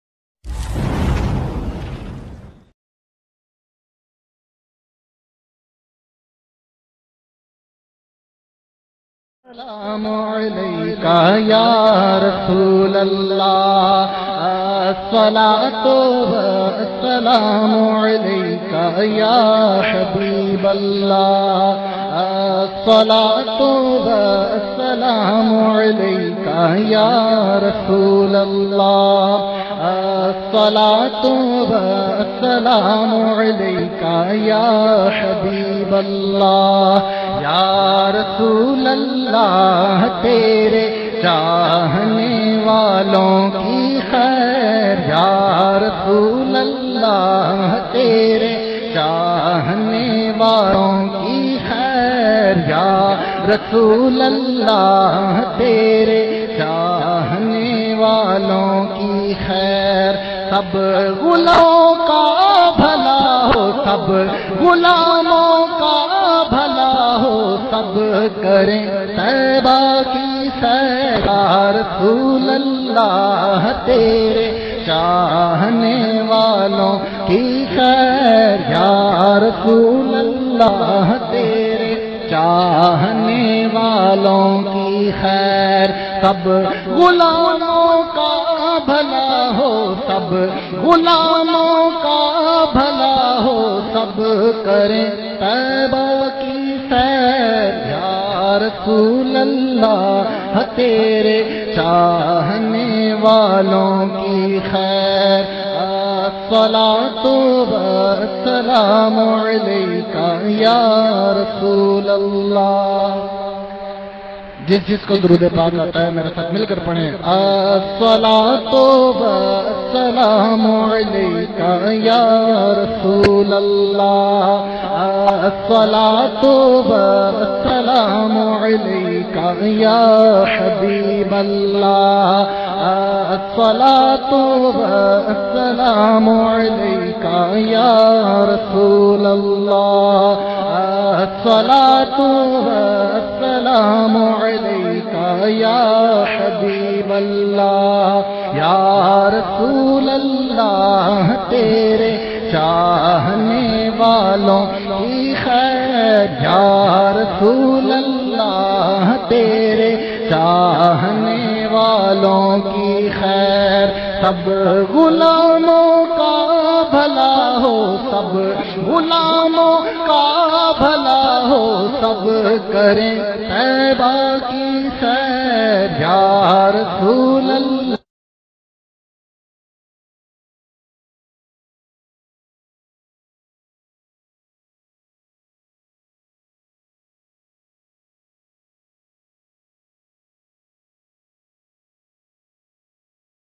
Ghazi Mumtaz Hussain Qadri Shaheed Naat In A Mehfil
Ghazi Malik Mumtaz Hussain Qadri Shaheed رحمة الله عليه Reciting A Beautiful Naat In A Mehfil,